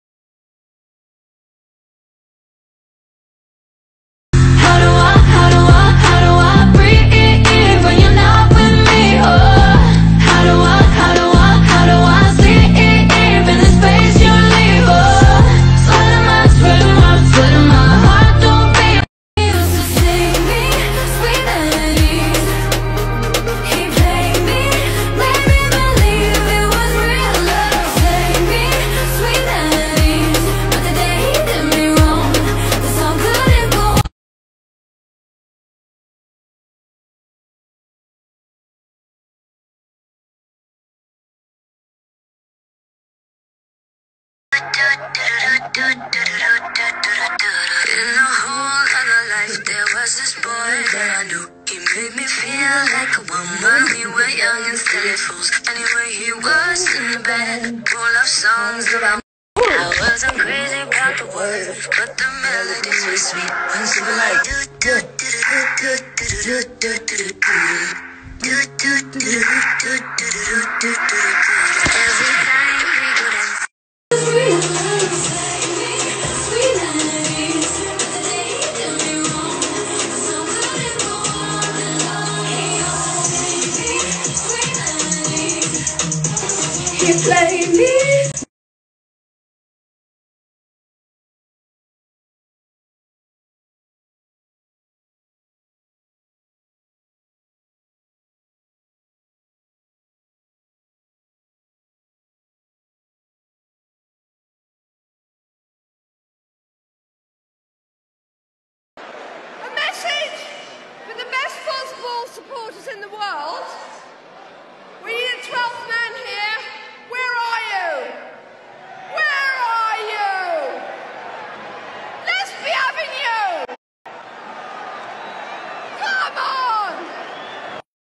Jade 11 novembre - Instagram 📱
Hey !

Voici la story de Jade le 11 novembre sur Instagram.

Liens qui pourrait t’intéresser ツ

+ d’infos sur moi ✍︎

✌︎ Je m’appelle Laura, j’ai 14 ans et je suis en 3ème !
✌︎ Je suis Mixers depuis juillet 2017.

MixersFR